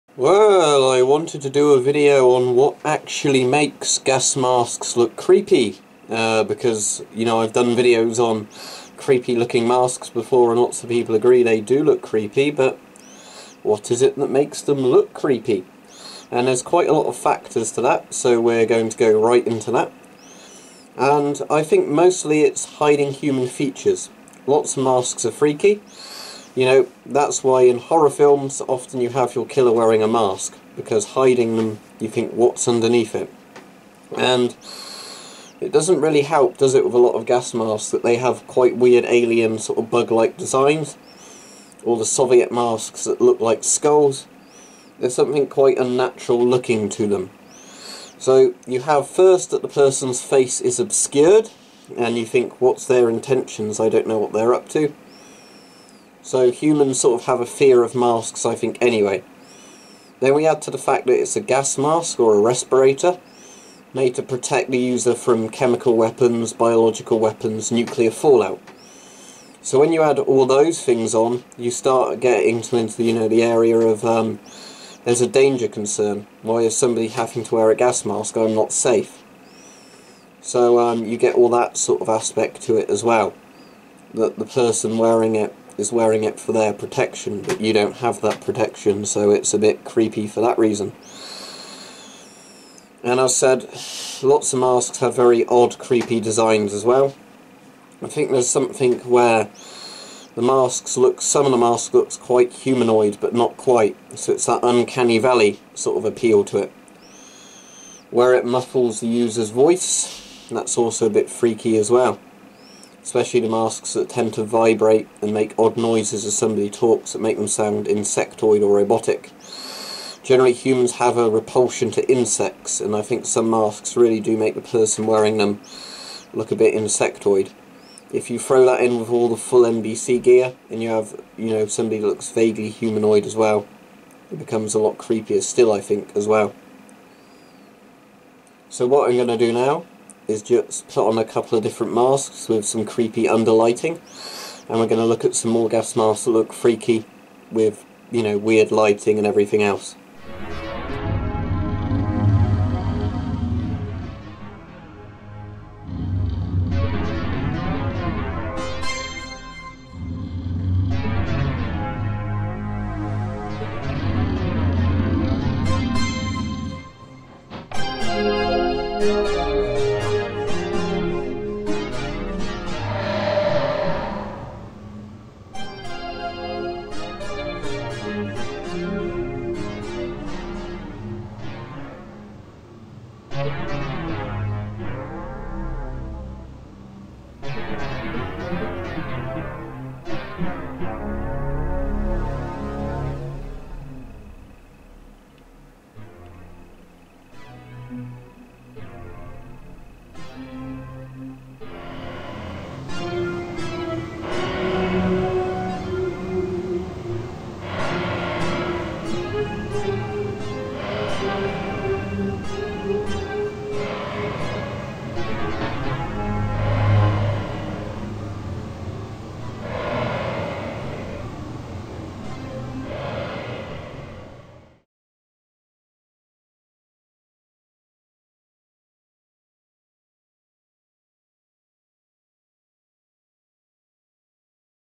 What makes Gas Masks Scary?
An explanation of the factors that make Gas masks or respirators scary to a lot of people.
These include the mask aspect that hides the wearers face, expression and intentions.
The danger the mask implies to those around the person wearing it.
The muffling or distortion of the wearer's voice.
The uncanny valley aspect of many of the masks that sort of resemble human faces but something seems off about them.
I also find that many respirators look insect-like, which adds to the repulsion factor to many observers.